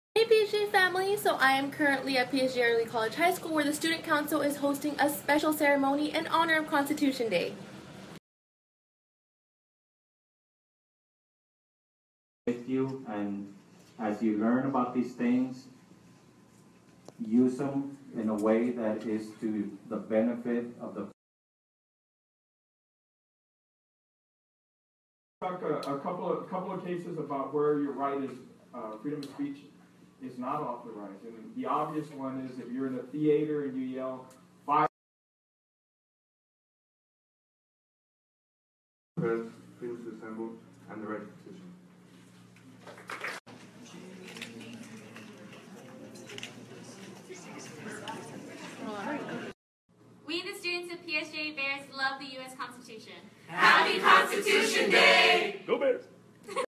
PSJA Snap Story: Constitution Day at PSJA ECHS
PSJA Early College High School's Student Council held a special Ceremony on Sept. 16 to celebrate Constitution Day.